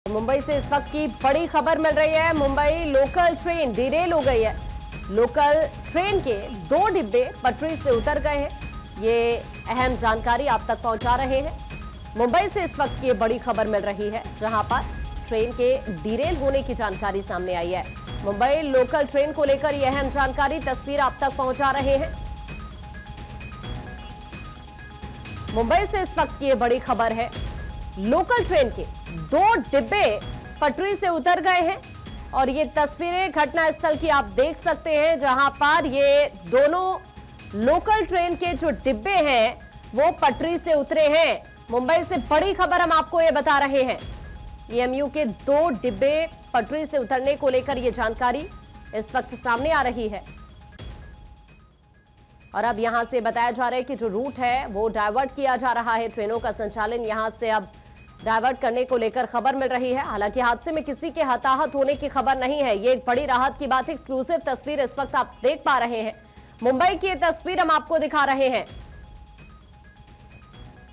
Breaking News: Mumbai local Train के दो डिब्बे पटरी से उतरे | Train Derail | Latest
मुंबई से इस वक्त की बड़ी खबर...  मुंबई लोकल ट्रेन हुई डीरेल... लोकल के दो डिब्बे पटरी से उतरे

Big news from Mumbai at this time... Mumbai local train derailed... two coaches of the local train derailed

Anchor Name : Akansha Rajput | Show Name : News | Time : 02: 30 PM | Date : 13/10/2024 | Published By: Vanshika